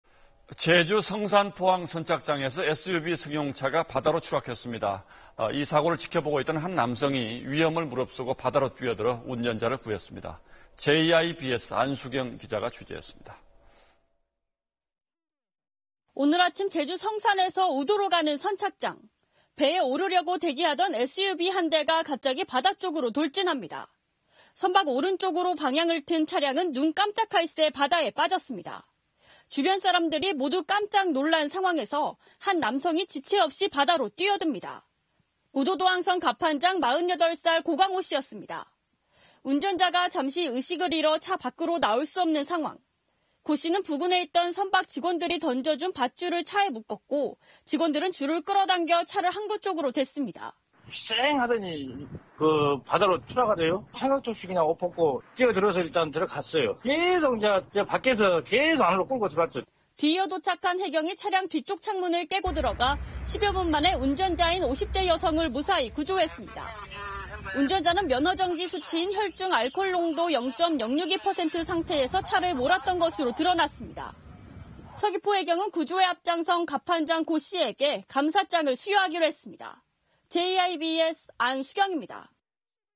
음주차량 바다 돌진…시민이 뛰어들어 구했다 / SBS
제주 성산포항 선착장에서 SUV 승용차가 바다로 추락했습니다. 주변에 있던 한 남성이 위험을 무릅쓰고 바다로 뛰어들어 운전자를 구했습니다.